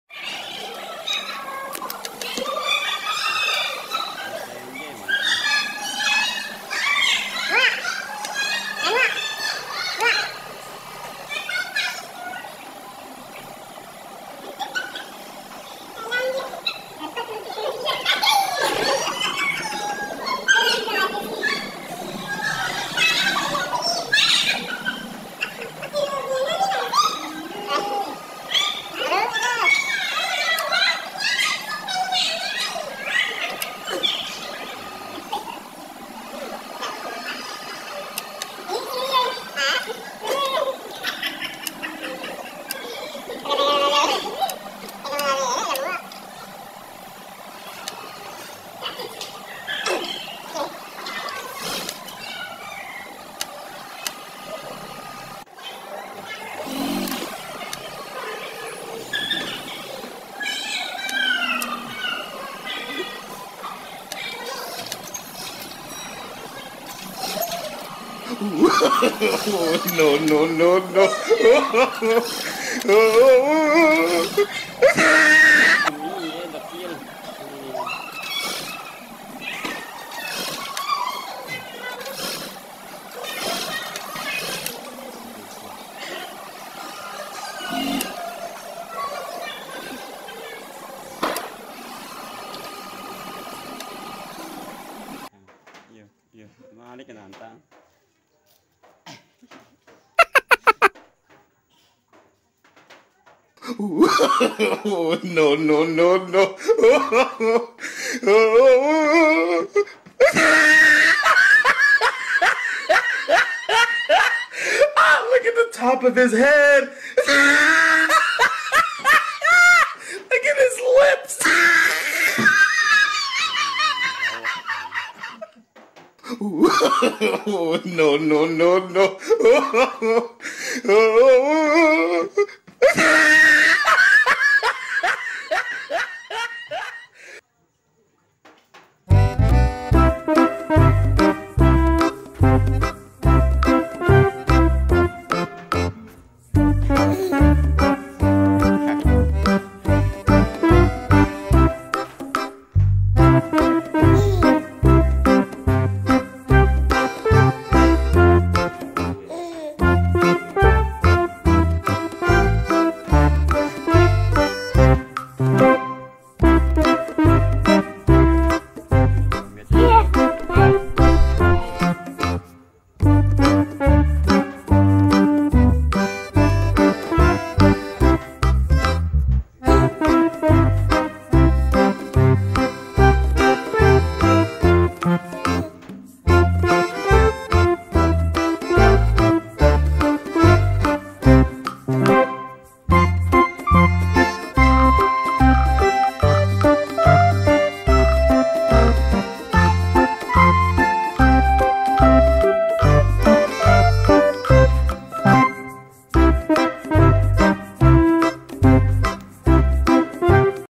kucing betina ngajak kawin si oyen,, namun si oyen masih malu malu kucing😆😆
kucing kawin
kucing lucu
kucing birahi
kucing
miong
meow meow